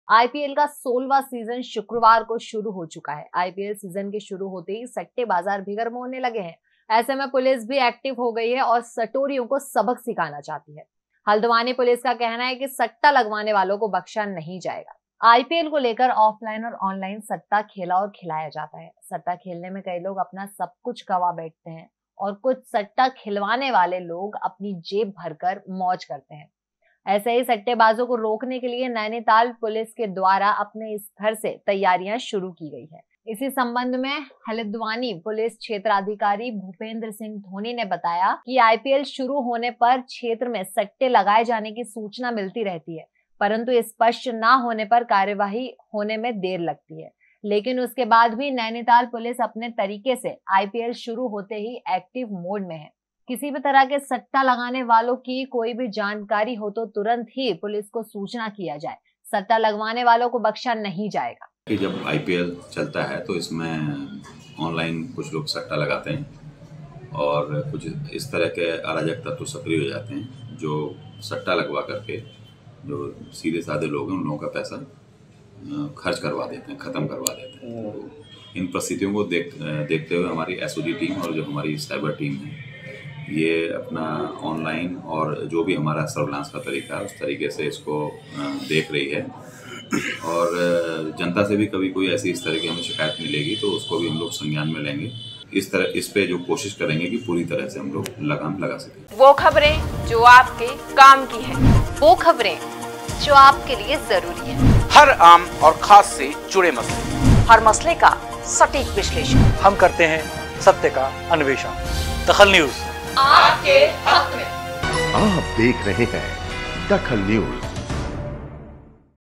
DAKHAL NEWS आईपीएल शुरू सट्टा बाजार गरमाया
आईपीएल शुरू सट्टा बाजार गरमाया
हर तरफ एक्टिव हो गए हैं सटोरिये

ipl 2023
ipl match
gambling
uttrakhand news
haldwani
madhyapradesh